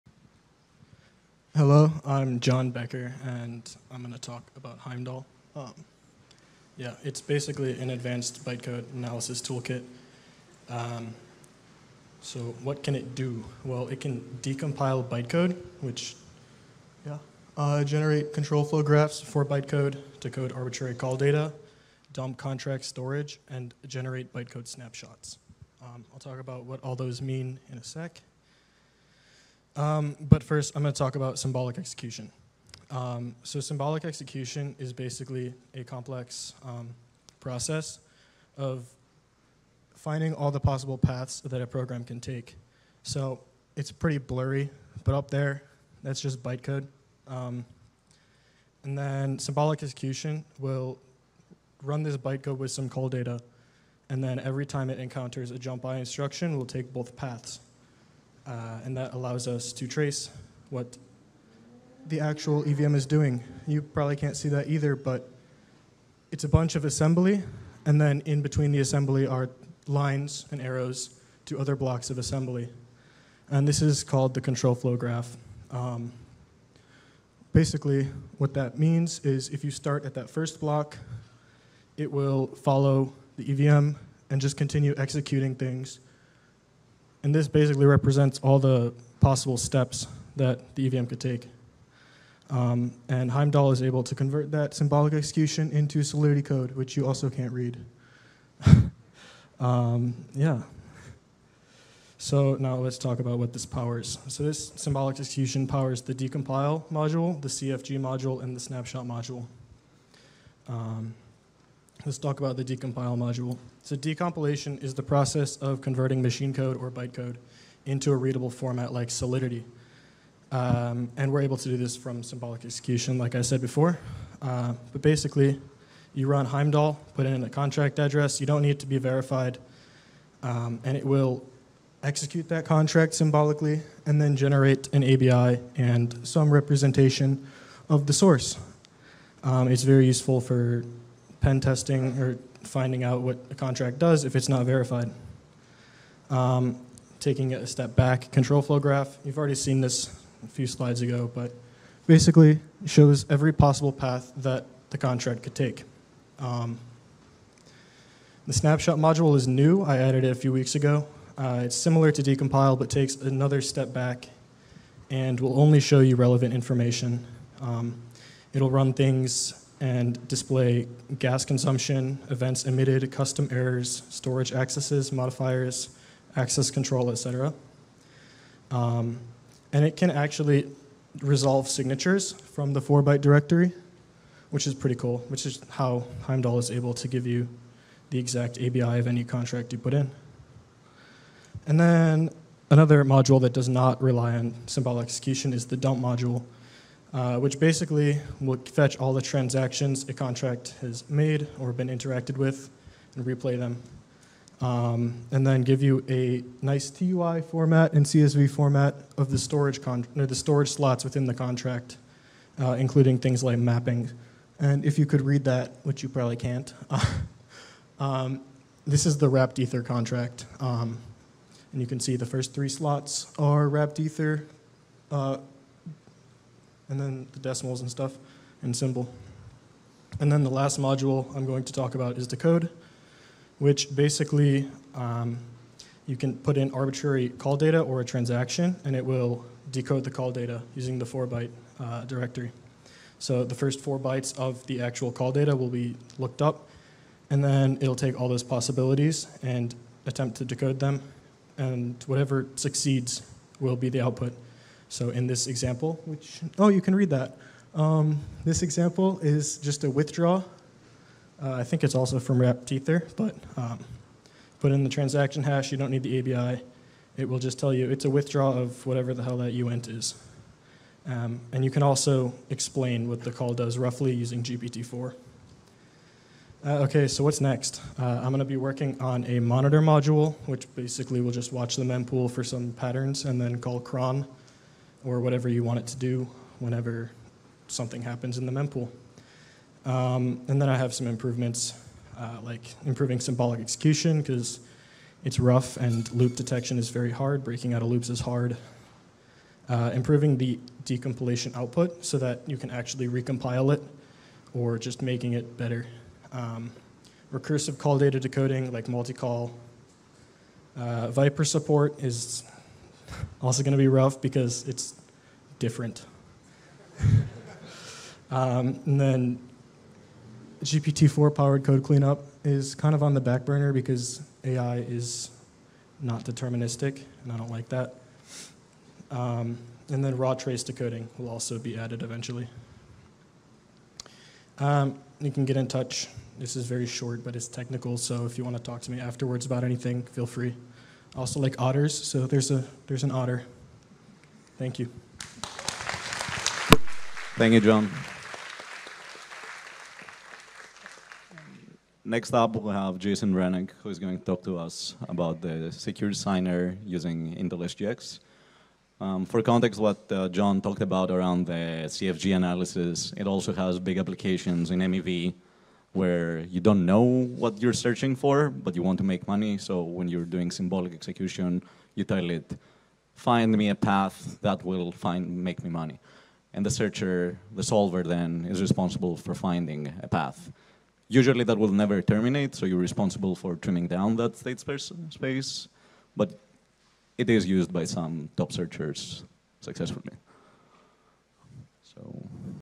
Heimdall, An Advanced Bytecode Analysis Toolkit @ Rust x Ethereum Day
At Paradigm's Rust x Ethereum Day in San Francisco on August 26, 2023, I talk briefly about Heimdall, a bytecode analysis toolkit for the EVM.